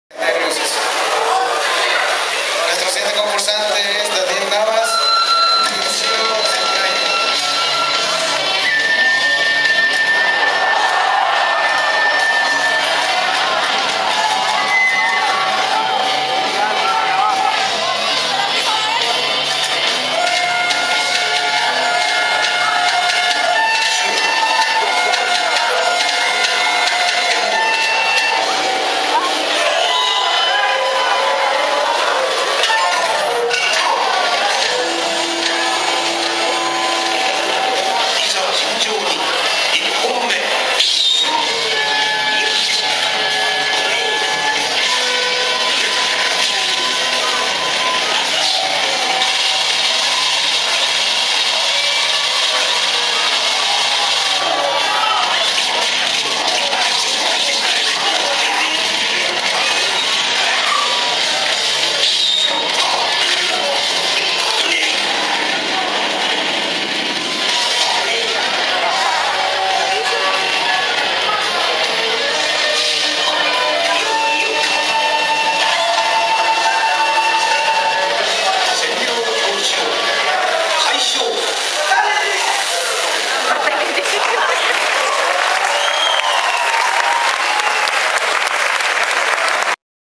Kurisumasu no Saiten Nana - Concurso de Cosplay Masculino - Samurai Showdown
Con el nombre oficial de la franquicia de Samurai Spirits, D. Navas interpretando a Kyoshiro.